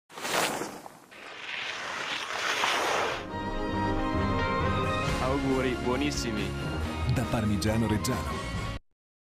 Auguri_buonissimi_Parmigiano Reggiano.mp4
Parmigiano Reggiano sceglie Giuliano Razzoli per la sua campagna pubblicitaria.